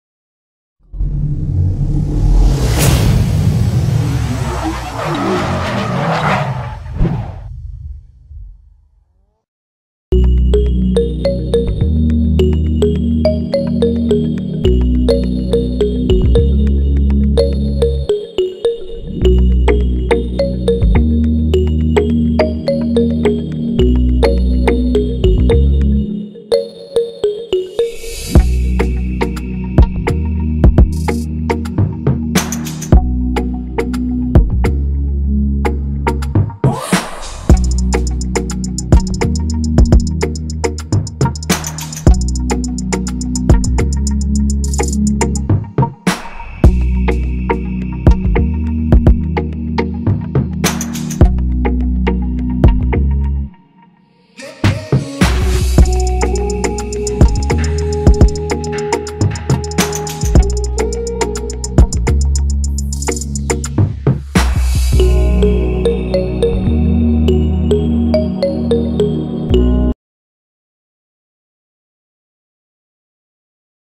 2023 DS 7 E-Tense Crossover Launched At 2022 Paris Motor Show -First look | Full Interior & Exterior
OFFCIAL WEBSITE AND PRODUCT DETAILS AND BOOKING :

DS Automobiles announced indicative prices for the DS 7 in France, with the configurator going live on the official website. Setting aside the entry-level 1.5-liter BlueHDi diesel engine producing 128 hp (96 kW / 130 PS), the facelifted DS 7 range is dominated by electrified powertrains. All of them are using the turbo 1.6-liter petrol as a base, combined with one or two electric motors, and a new 14.2 kWh battery offering up to 65 km (40 miles) of EV-only range. The FWD-only E-Tense 225 has a combined output of 222 hp (166 kW / 225 PS), while the E-Tense 4×4 300 steps up the game with dual electric motors for a combined 296 hp (220 kW / 300 PS). On top, there’s the E-Tense 4×4 360, which brings the more impressive figure of 355 hp (265 kW / 360 PS). The latter accelerates from 0-100 km/h (0-62 mph) in 5.6 seconds, and benefits from a model-specific chassis setup by DS Performance, including lowered suspension, wider tracks, and larger brakes.